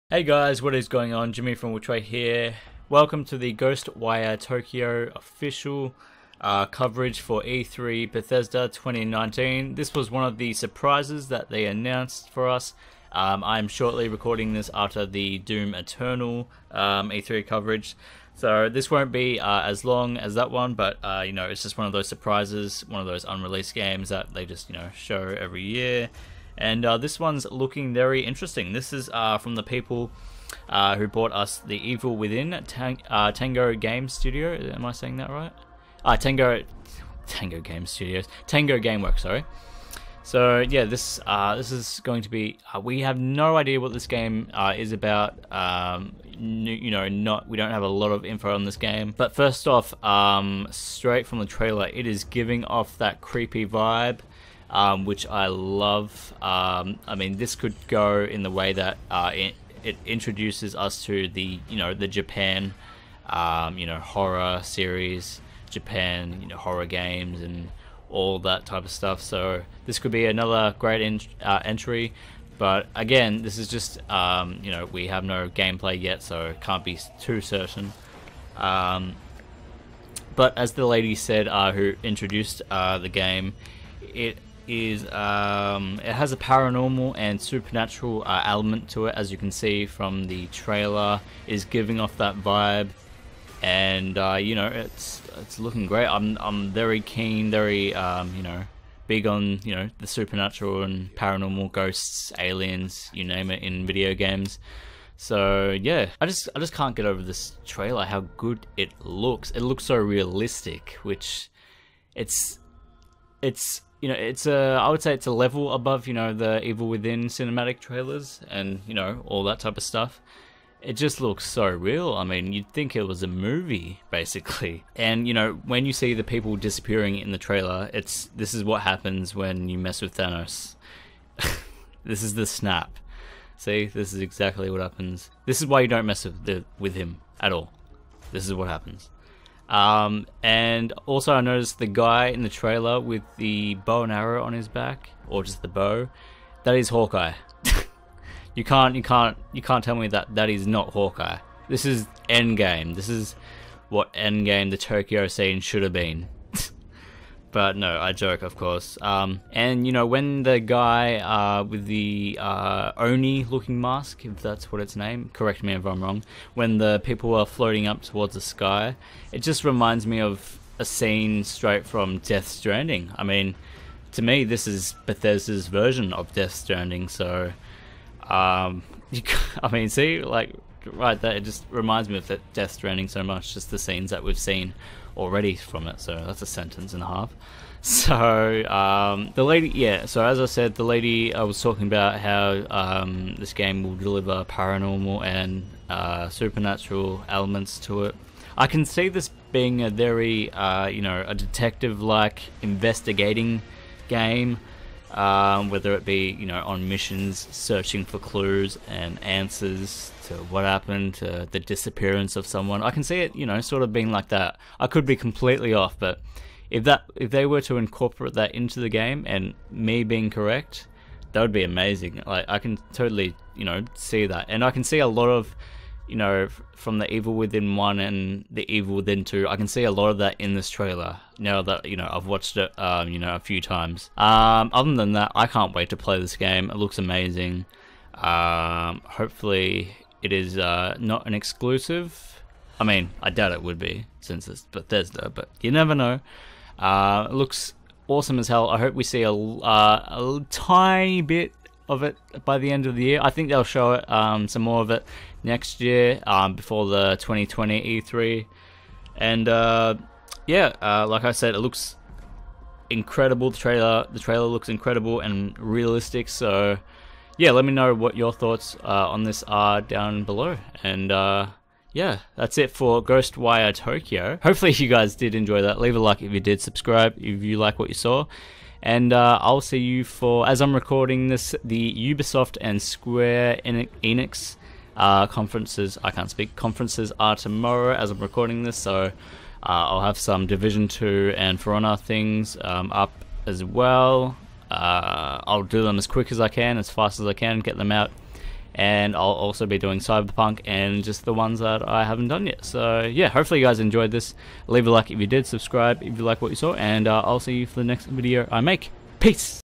E3 2019 Commentary: Ghost Wire Tokyo! (Thoughts+Info)
Hey guys, Jimmy from WhichWay here!  Bethesda dropped this one as a surprise during their conference, Ghost Wire Tokyo. This is another game that has a fantastic cinematic trailer. Can't wait to see more on it. Let me know your thoughts about it down below. Hope you guys enjoy!